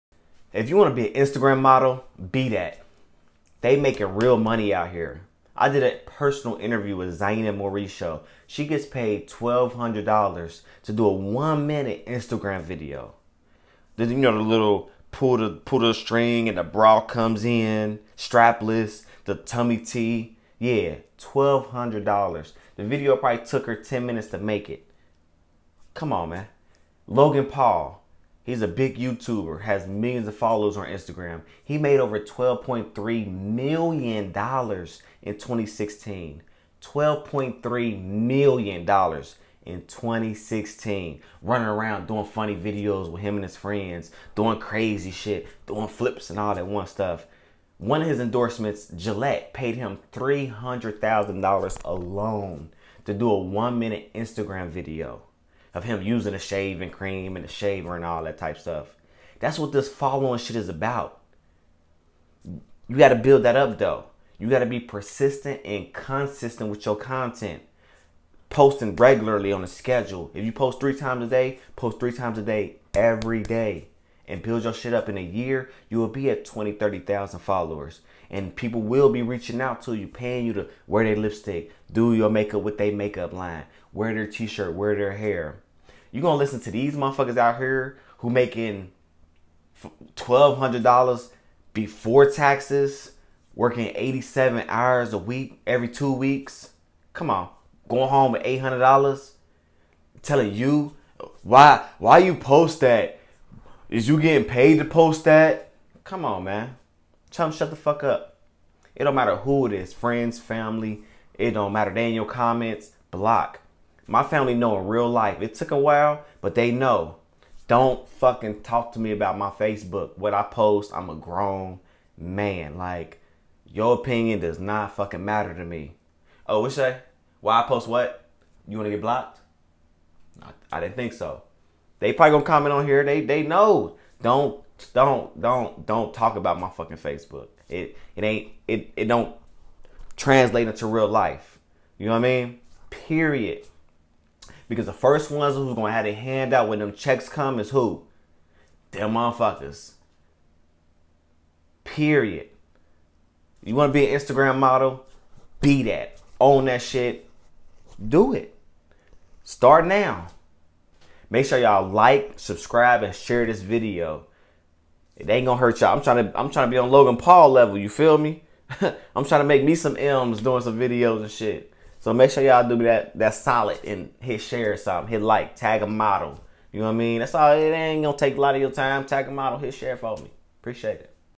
How to become an Instagram model
Instagram models get paid A LOT of money. More than most people can imagine.
Getting paid $1,200 for a 1-minute instagram video for some is a DREAM... But for Zaena Morisho and others, it’s their reality...